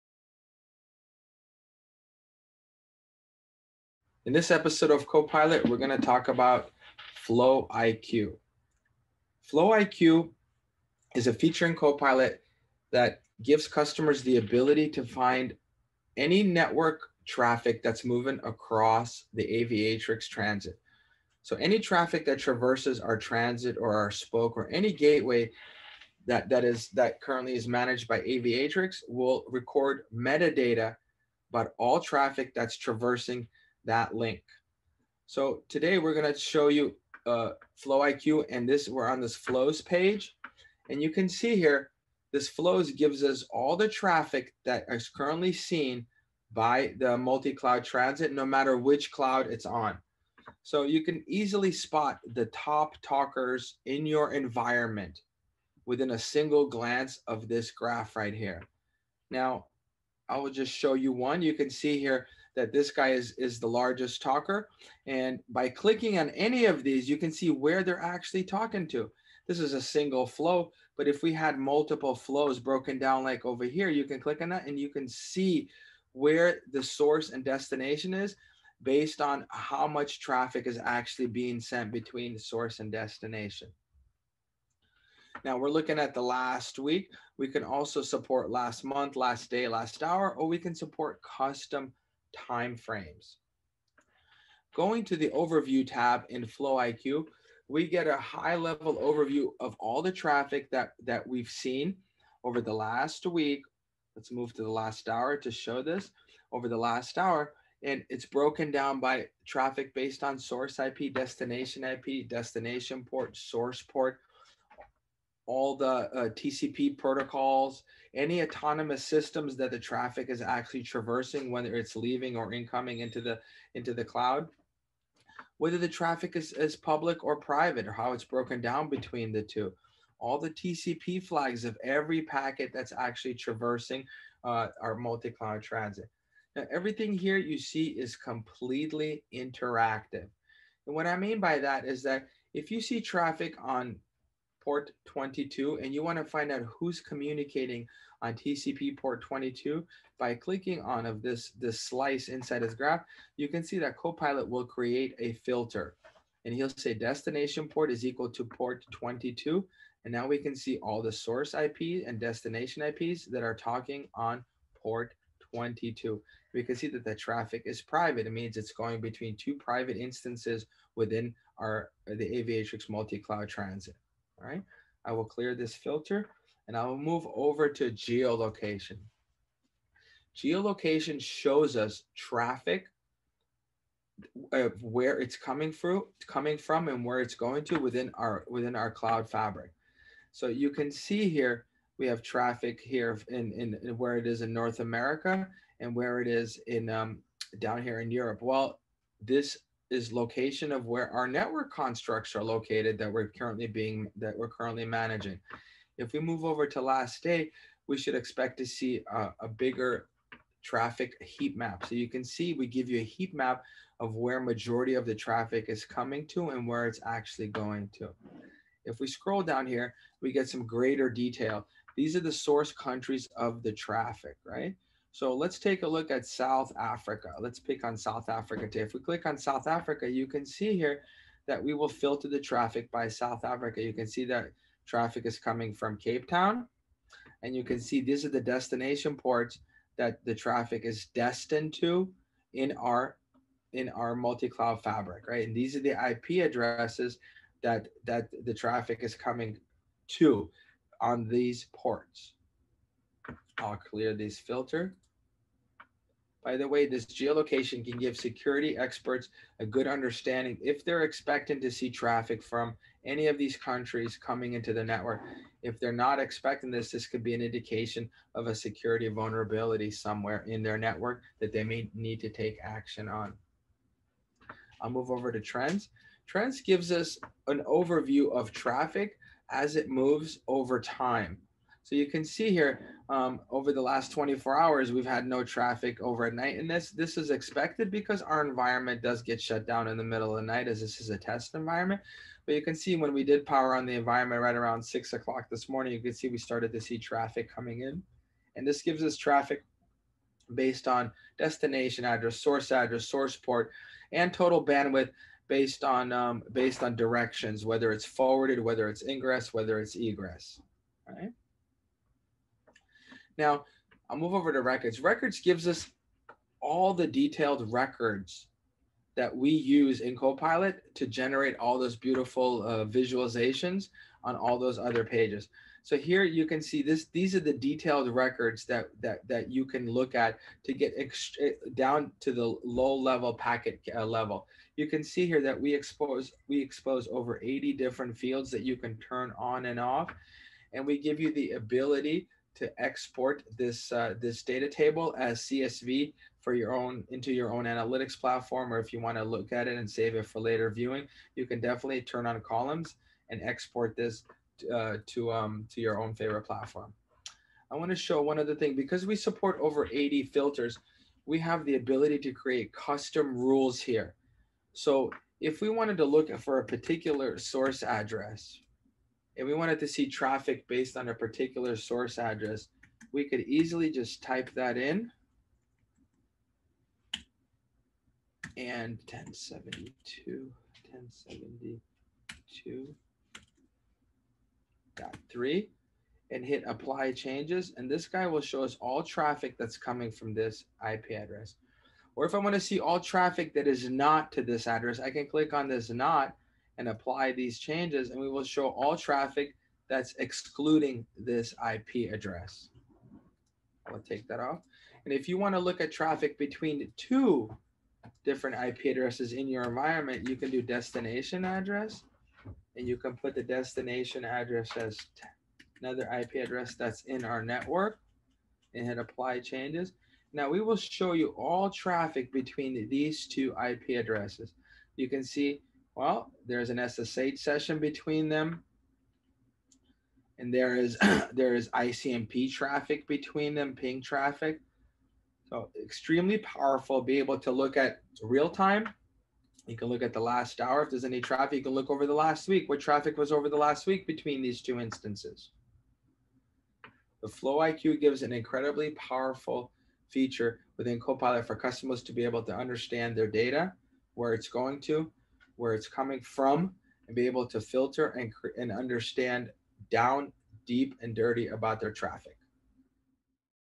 E5 Aviatrix CoPilot FlowIQ
Visualize every network flow across your multi-cloud network and drill down into the details